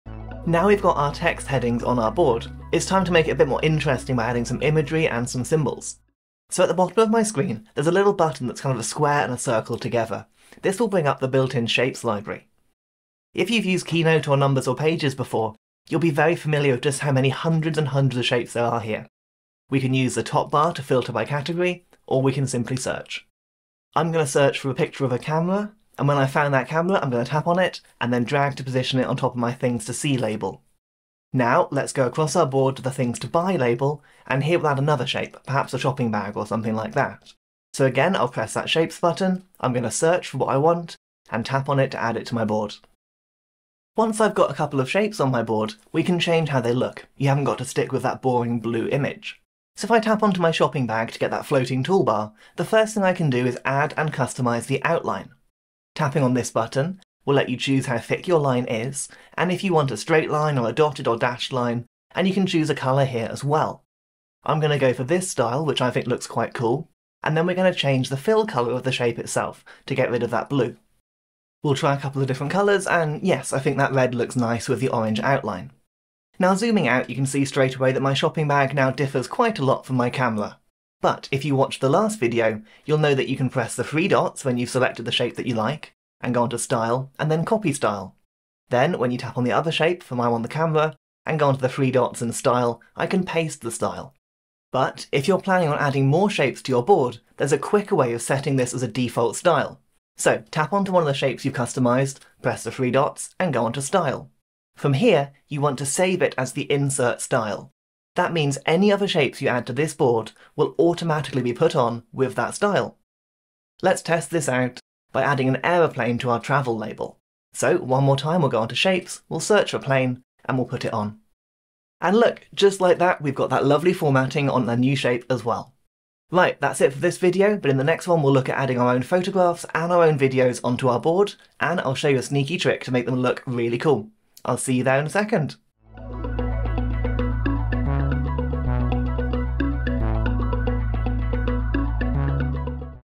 Freeform: Shapes and Custom Shape Styles | Complete Guide for iPhone (3/9)
📱 Shapes and Symbols can really bring a Freeform Board to life - especially once you know how to Copy + Paste Styles and change the Default Style for new shapes.  That means you can quickly and easily create great looking graphics that'll help you better organise your Freeform Boards and hopefully enhance your productivity.